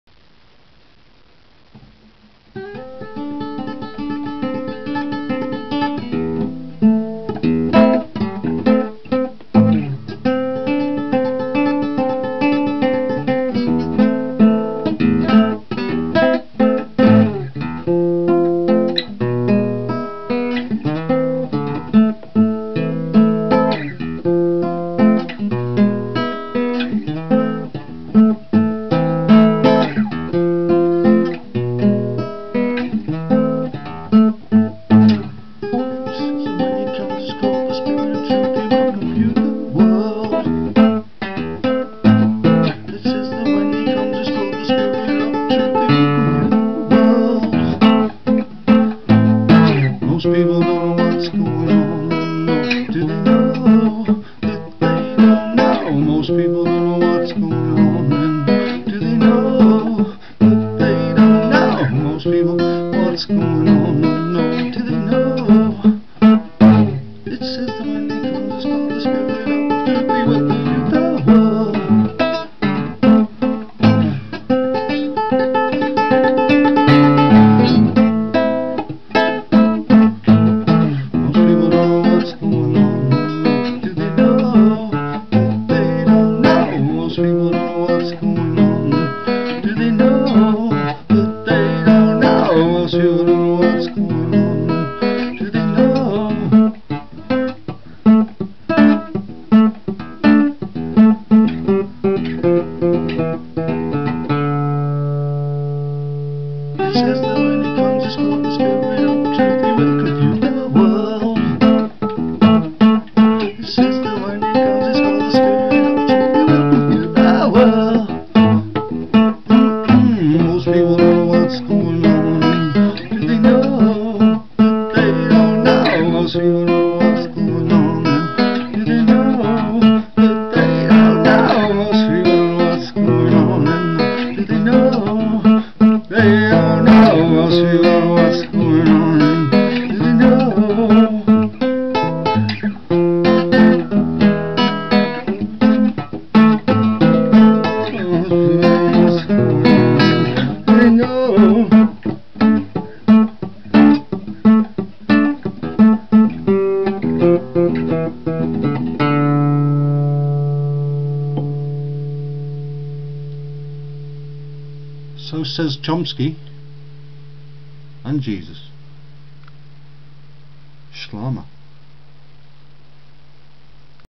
MOST PEOPLE DON'T KNOW WHAT'S GOING ON, NOR DO THEY KNOW THAT THEY DON'T KNOW
It says that when he comes whos called The Spirit of Truth
he will confute the world.

Most people dont know whats going on ,
nor do they know they dont know. :)

john 16